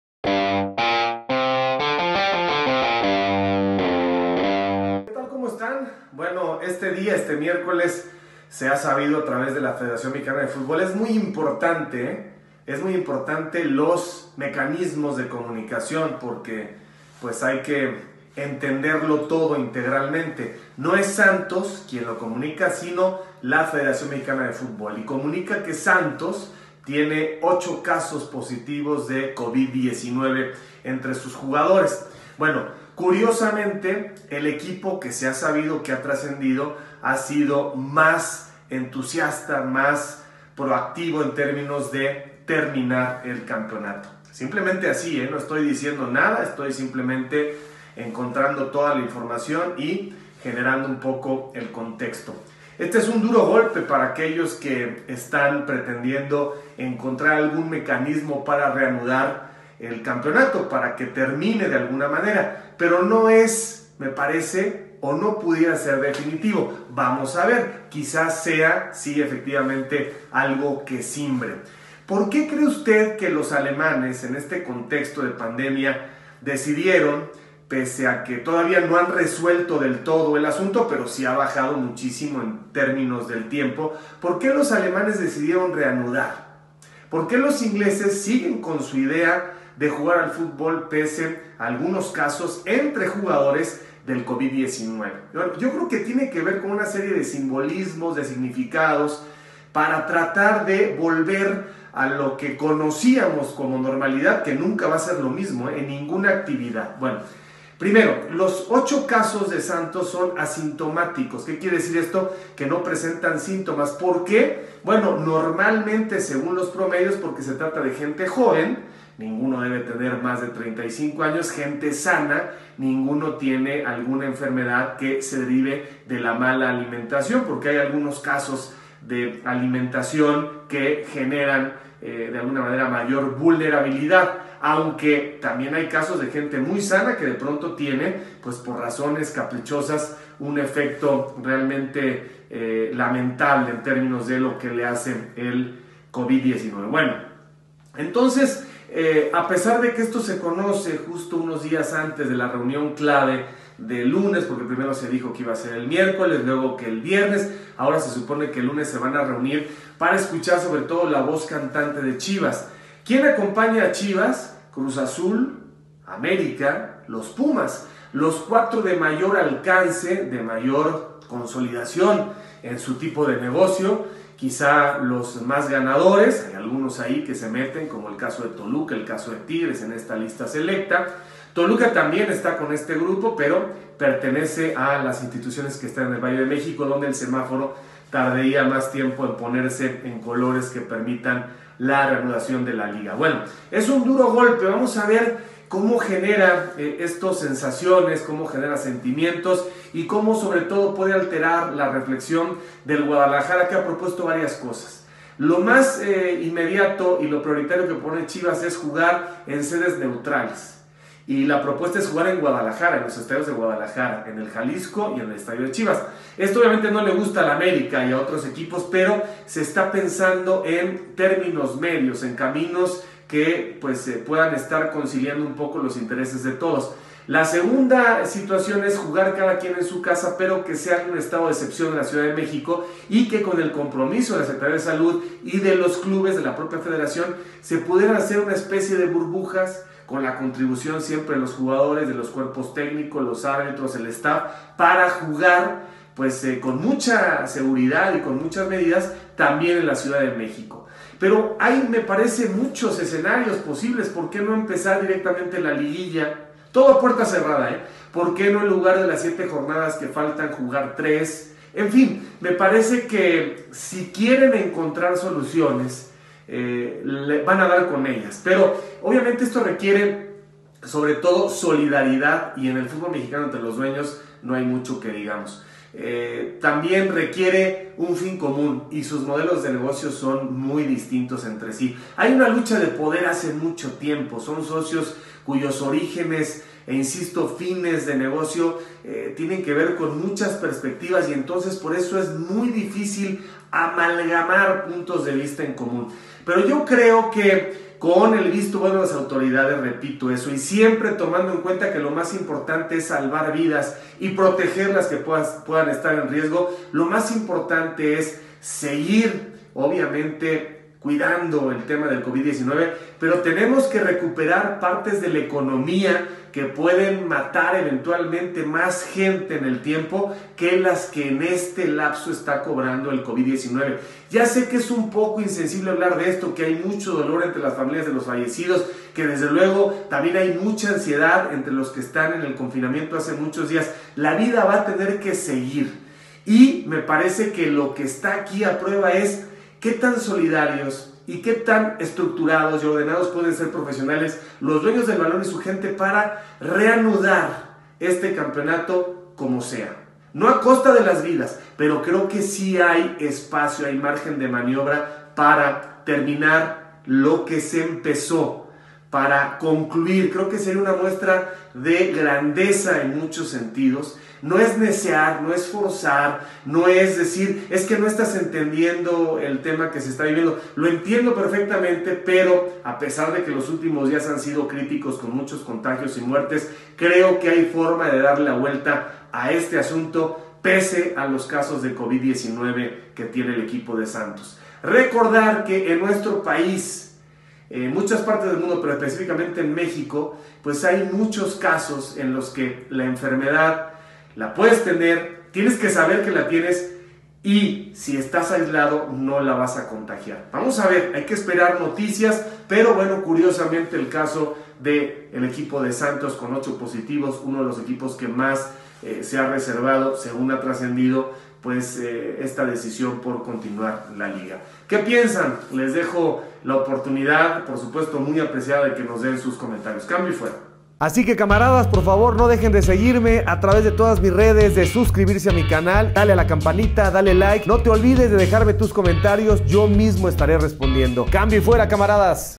8 COVID en Santos ¿Qué cambia? | Javier Alarcón
Hay 8 casos de COVID19 en el equipo de futbol de SANTOS ¿Eso cómo afecta al futbol mexicano?